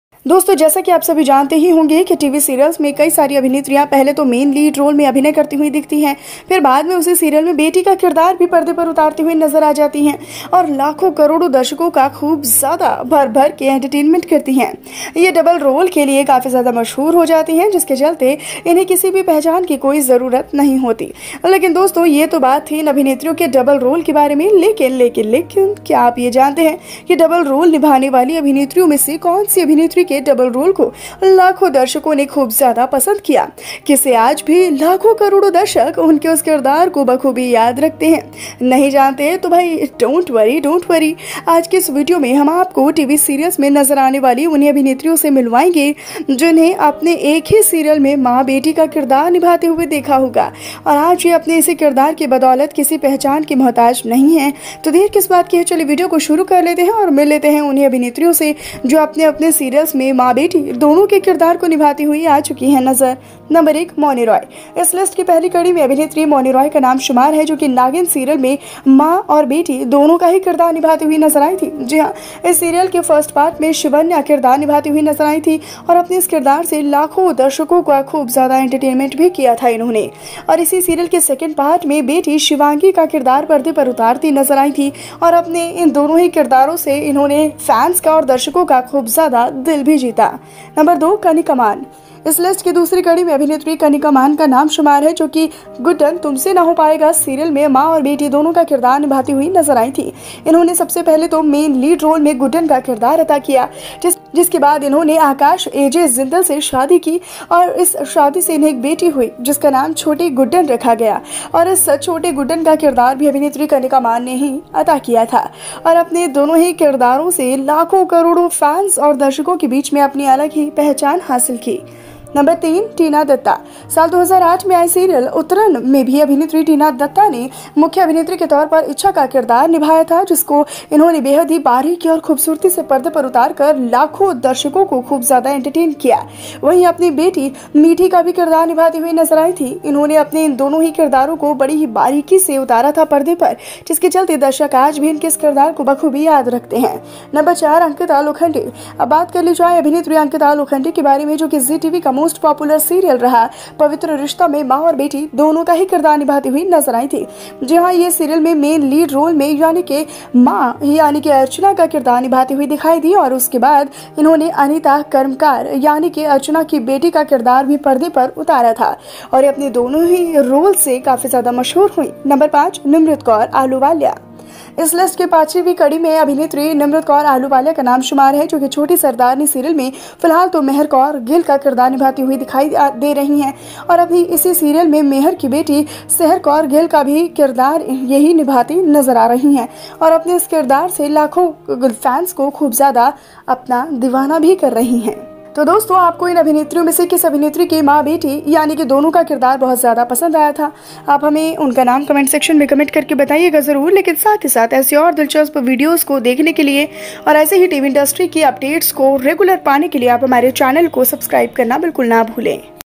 एक ही सीरियल में मां बेटी दोनों के किरदार निभाए टीवी इंडस्ट्री की इन मशहूर अभिनेत्रियों ने
These_ famous_ actresses_ of_ the_ TV_ industry_ played_ the_ roles_ of_ both_ mother_ and_ daughter_ in_ the_ same_ serial

Title: This actress appeared in the role of both mother and daughter in the same serial..

 Friends, as you all know that in TV serials, many actresses are first seen acting in the main lead role, then later in the same serial, the daughter's character is seen on the screen, and millions of crores of viewers are entertained a lot.  She does, and she becomes quite famous for her double role, due to which she does not even need any identity!

But friends, this thing was about the double role of these actresses, but do you know which of the actresses who played double roles, the double role of which actress was well-liked by millions of viewers...? And who even today millions and crores of viewers remember very well in that character. , otherwise it doesn't matter, because in today's video we are going to introduce you to the same actresses seen in TV serials, who Have seen mother playing the role of daughter in the same serial and today she is not interested in any identity due to this character. , so what's the delay, let's start the video, and meet the same actresses Who was seen playing the role of mother daughter in her serial!

 No. 1 Mouni Roy

 The name of the actress "Mouni Roy" is included in the first episode of this list, who was also seen playing the role of both mother and daughter in the serial "Naagin". Yes it is "Shivanya" in the first part of this serial.  She was seen playing the character, and had also entertained millions of millions of viewers with her character, and in the second part of this serial, the daughter was seen portraying the character of "Shivangi", and millions from both her characters.  Millions of spectators were entertained a lot!

 Number : 2 Kanika Mann

 In the second episode of this list, the name of the actress "Kanika Mann" is included * who was seen playing both mother and daughter in the serial "Guddan Tumse Na Ho Payega", she first played "Guddan" in the lead role  After which she married "Akash AJ Jindal", and from this marriage she had a daughter, who was named "Chhoti Guddan", and the character of this "Chhoti Guddan" was also actress "  Kanika Mann" played, and with both her characters, she made a different identity among millions of viewers!

 No. 3 Tina Dutta

 In the serial "Uttaran" in the year 2008, actress "Tina Dutta" played the character of "Ichha" as the lead actress, which she had entertained millions of crores of viewers by performing very beautifully on the screen, she was her daughter.  She was also seen playing the character of "Meethi", and she nailed both her characters so closely that the audience still remembers her character very well!

 No. 4 Ankita Lokhande

 Now let's talk about the actress "Ankita Lokhande", who was also seen playing both mother and daughter in Zee TV's most popular serial "Pavitra Rishta".  She was seen playing the role of mother i.e. "Archana", and after that she also appeared on the screen as "Anita Karmakar" i.e. Archana's daughter, and it became famous with both her roles.  !

 No.5 Nimrit Kaur

 In the fifth episode of this list, the name of actress "Nimrit Kaur Ahluwalia" is included, who is currently seen playing the role of "Mehar Kaur Gill" in the serial "Choti Sarrdaarni", and now the daughter of "Mehar" in the same serial.  She is also seen playing the same character of "Sehar Kaur Gill", and is winning the hearts of millions of viewers with her character!

So friends, which of these actresses did you like the character of mother-daughter of the actress very much. ? You must tell us their name by commenting in the comment box, as well as you do not forget to subscribe to our channel to know more such funny news related to the TV world!